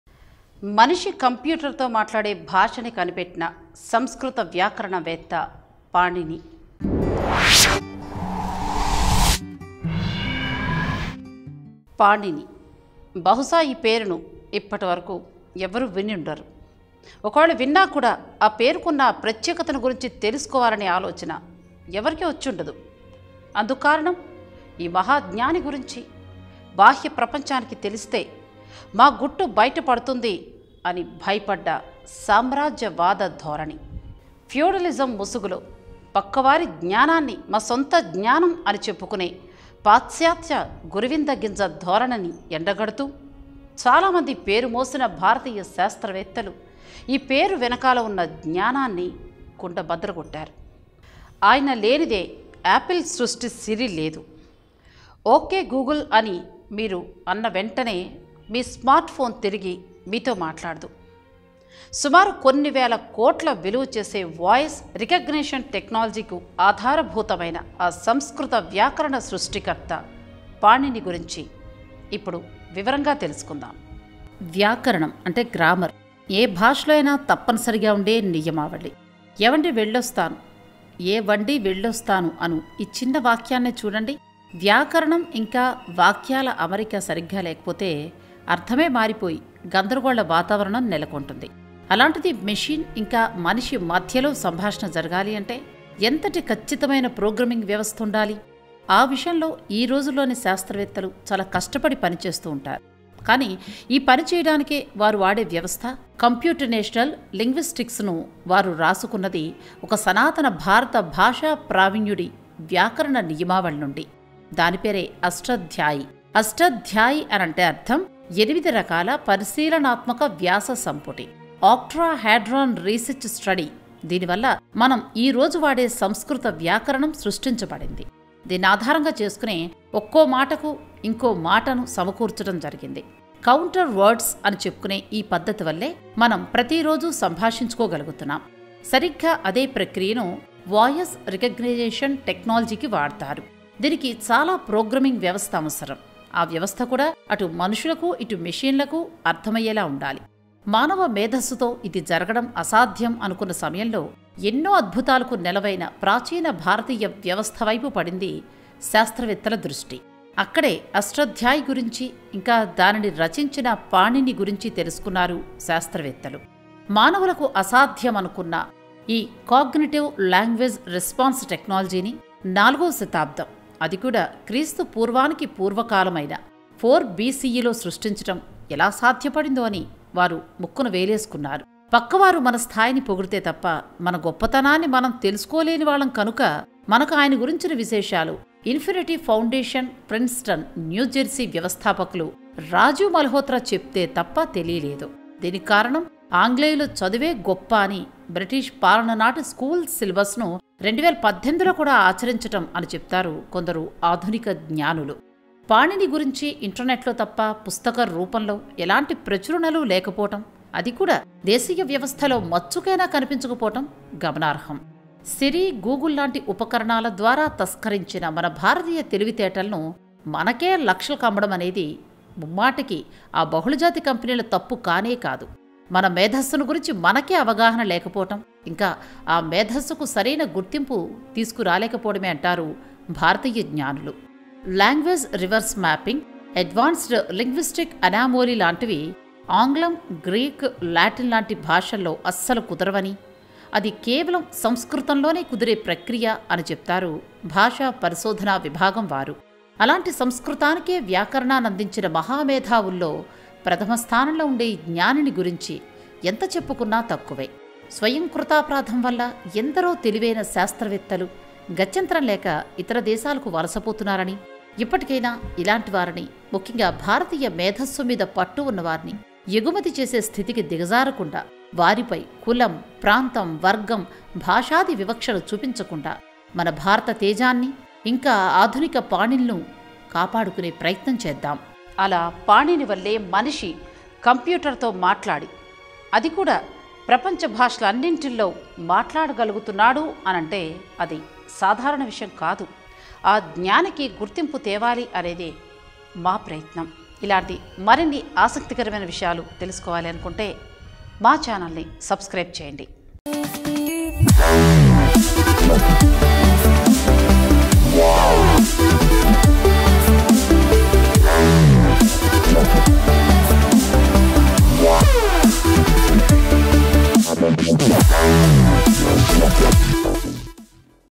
మనిషి కంప్యూటర్ తో మాట్లాడే భాషను కనిపెట్టిన పాణిని | The Vedic Father of Computational Linguistics.
Watch: మనిషి కంప్యూటర్ తో మాట్లాడే భాషను కనిపెట్టిన పాణిని..! | The Vedic Father of Computational Linguistics..! | Eyecon Facts

Computational  Linguistcs , The Intermediary Language which holds a vast amount of data repository and acts as a communication medium between the Human & Machine Interface finds its deep rooted ruins from the vedic period, Here We Present You the amazing story of a 6th Century BCE Grammar & Linguistic Expert Panini, Who is considered as the father of computational linguistics but as usual didn't find his way to  limelight , Thanks to the Double Standards of the Western World, Indian Technological Brilliance has been ignored once again. Be rest Assured as You are in the right  place for Knowing the Lost glory of Indian Vedic Wisdom dominating the computer world.

Eyecon Facts.